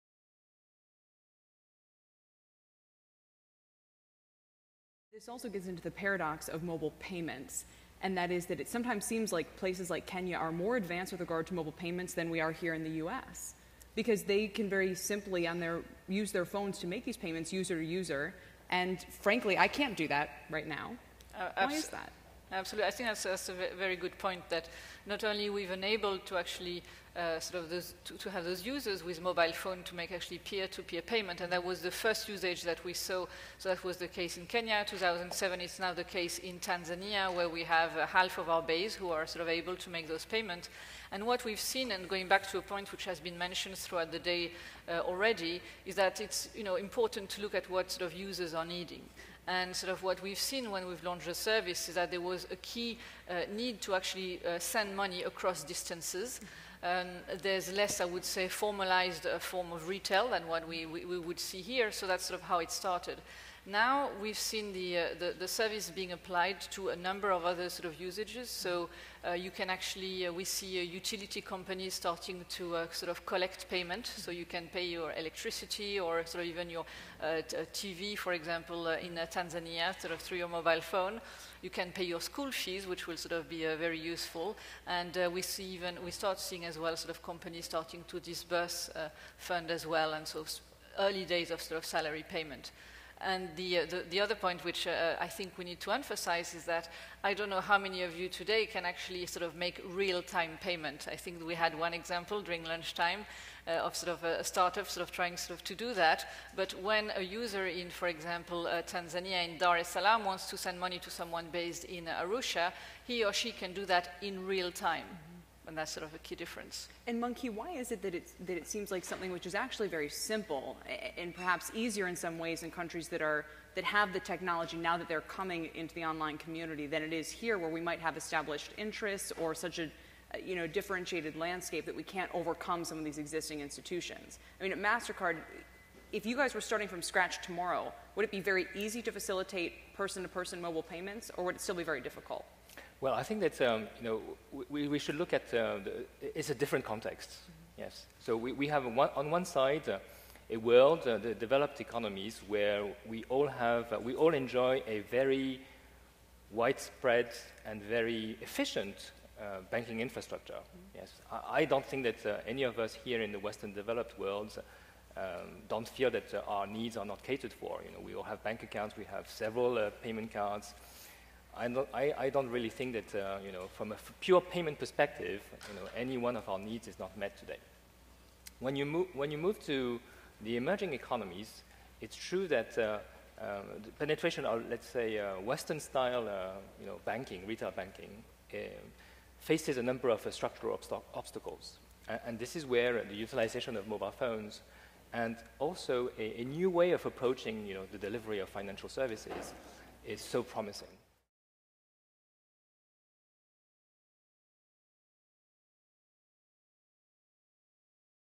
Mobile Payment Systems In Emerging Markets
Why do mobile payment systems sometimes seem better in emerging markets, like Kenya, than they do in Western markets like the United States? Mung Ki Woo of MasterCard and Claire Alexandre of Vodafone discuss.